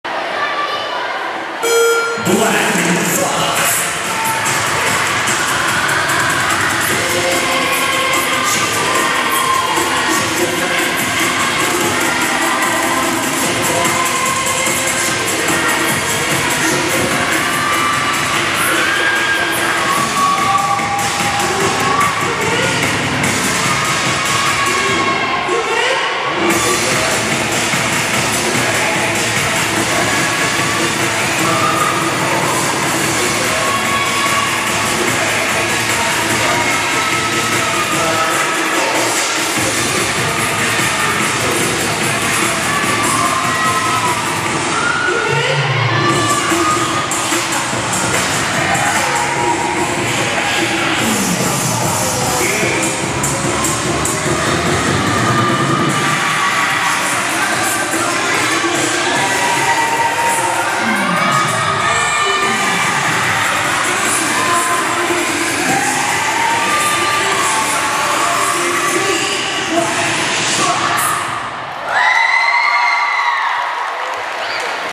🏆 WinterCup2023 🏆дуэты
🔥Чемпионат по современным танцам 🔥

🗓️Даты проведения: 25-26 февраля
📍Место проведения: г. Молодечно, ул. Великий Гостинец, 102, «Спортивно- развлекательный центр «Олимпик-2011»

Танцевальные направления:
⭐️Хип-хоп